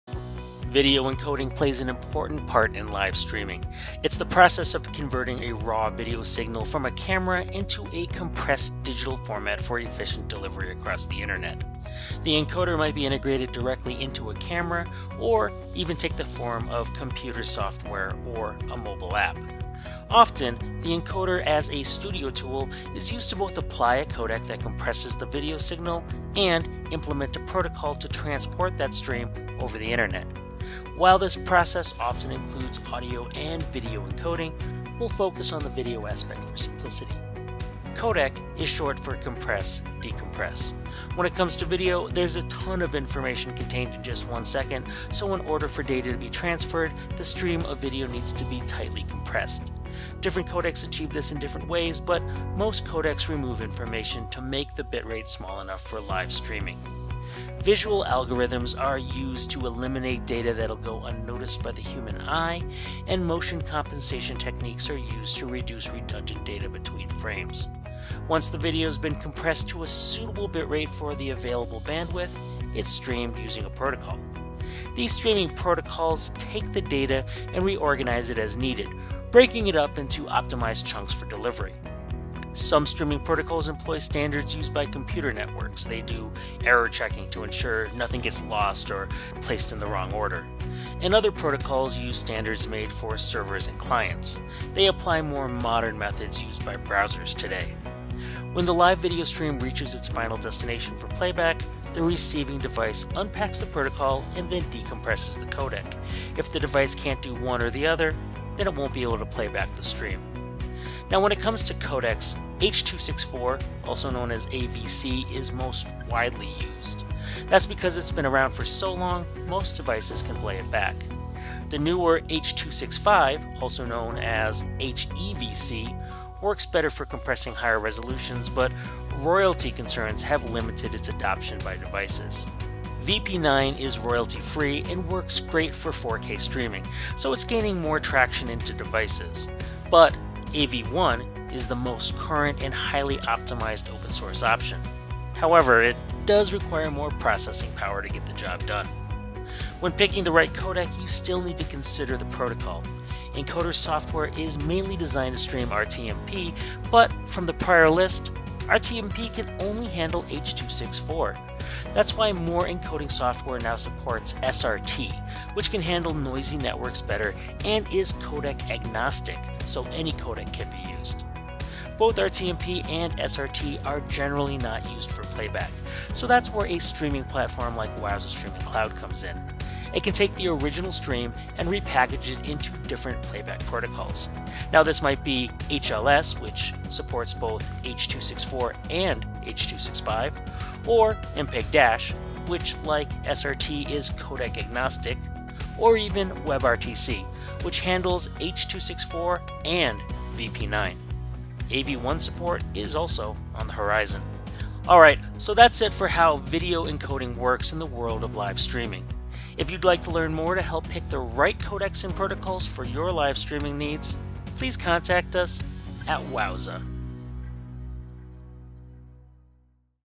How Does Video Encoding Work?
Video encoding plays an important part in live streaming. It’s the process of converting a raw video signal from a camera into a compressed digital format for efficient delivery across the internet. The encoder might be integrated directly into a camera, or even take the form of computer software or a mobile app. Often, the encoder as a studio tool is used to both apply a codec that compresses the video signal and implement a protocol to transport that stream over the internet. While this process often includes audio and video encoding, we’ll focus on the video aspect for simplicity. for more on video encoding.